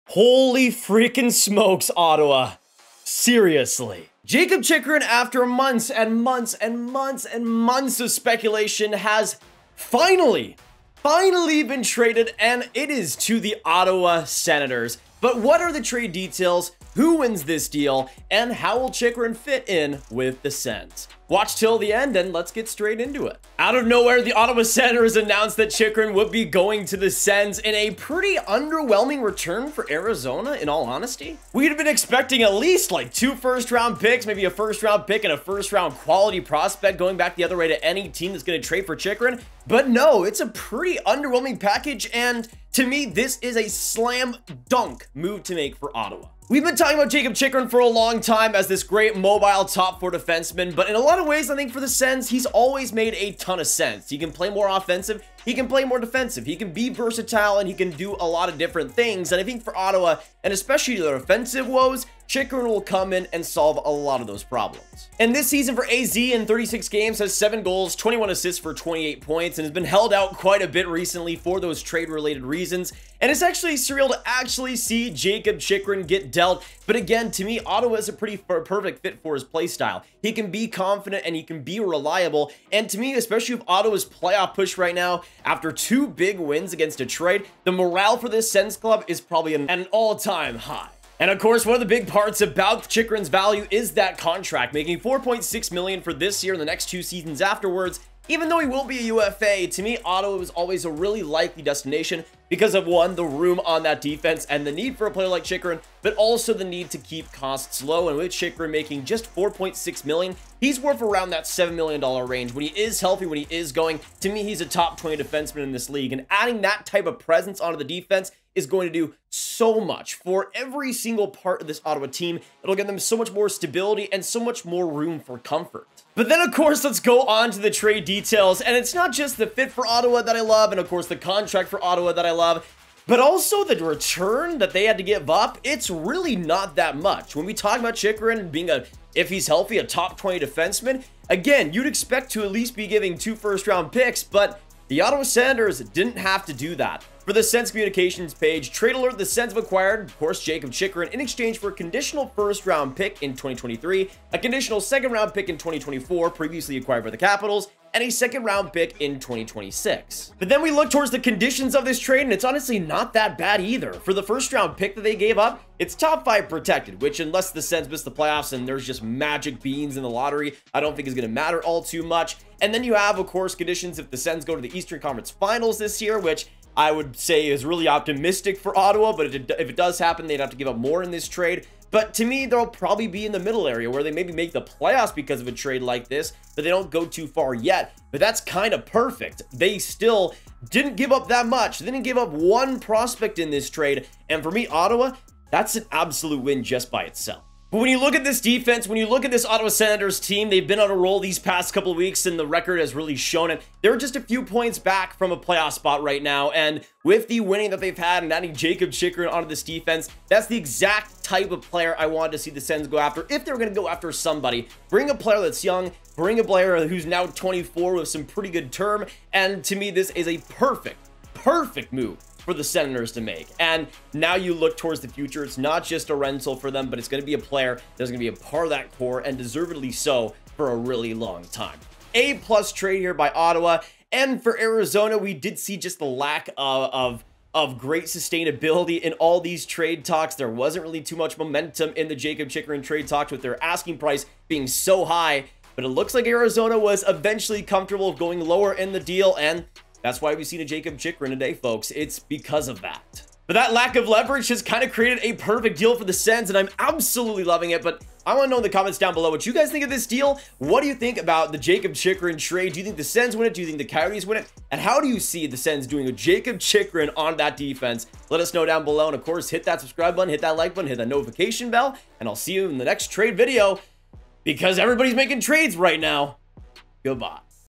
Coyotes TRADE Jakob Chychrun to Ottawa Senators: Who Won The Deal?
Nathan "Grav" Murdock  @GravitehHockey  breaks down the trade between the Ottawa Senators and Arizon Coyotes that sees defenceman Jakob Chychrun dealt to Ottawa. What do you think?